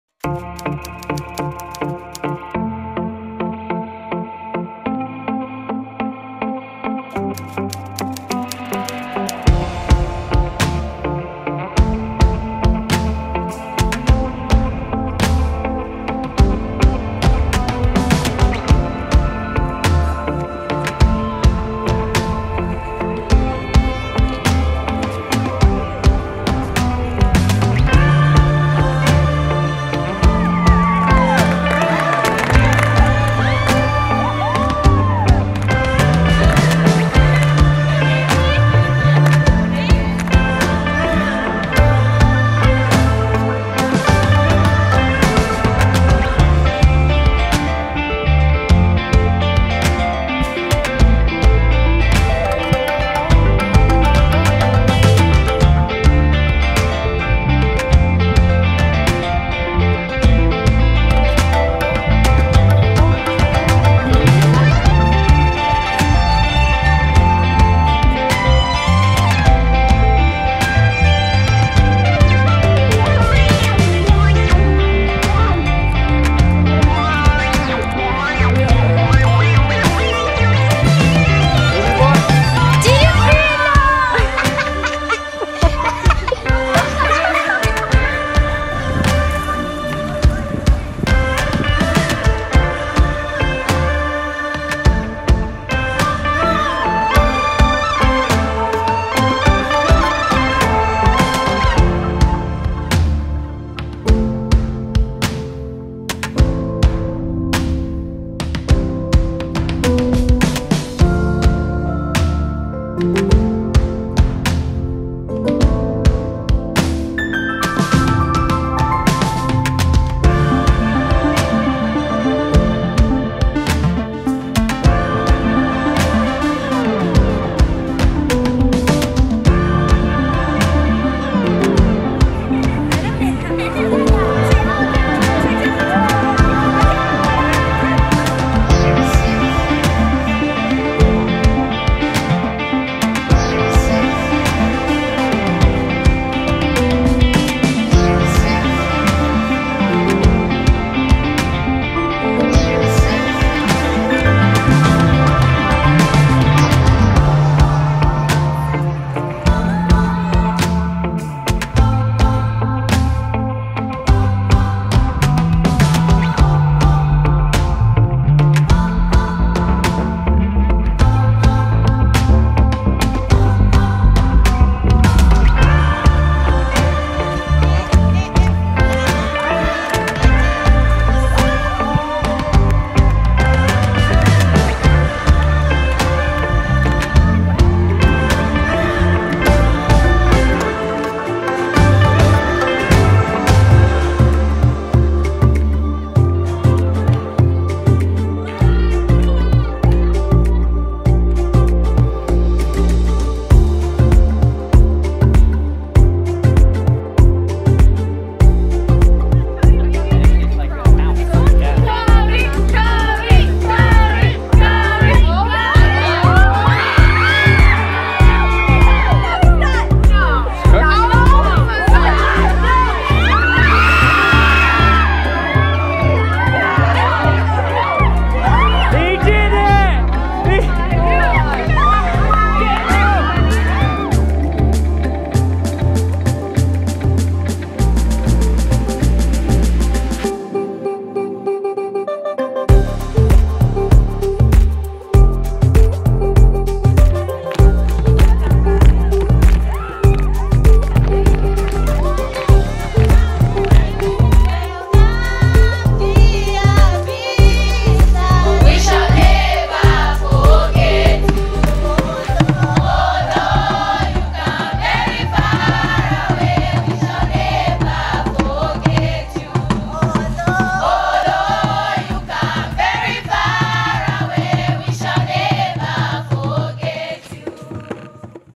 ZAMBIA VLOG || Part 1
We were so lucky to be able to attend the Somewhere Devine Expedition this year with Hailey and Brad Devine, and an AMAZING group of people. Thank you to I am Zambia who gave us life changing projects that in turn, made us better people.